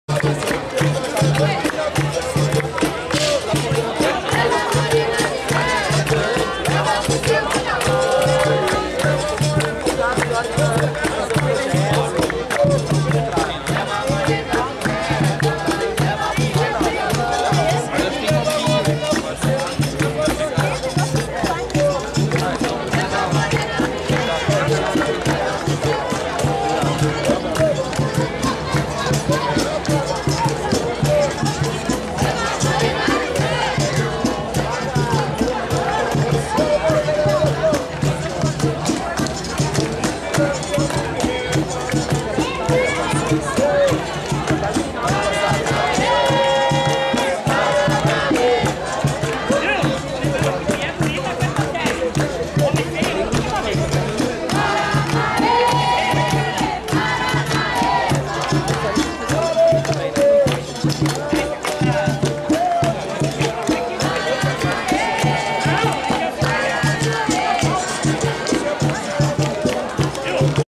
M2U00665 Capoeira
Abertura da Semana Zumbi Palmares com o apoio do Clube Lions Batel, FAE, UFPR, COPEL, SANEPAR, Prefeitura de Colombo, etc.  e da Associação Recreativa e Esportiva Zumbi dos Palmares.
O Clube Lions Batel comemora seu Jubileu de Ouro, 50 anos a serviço da comunidade.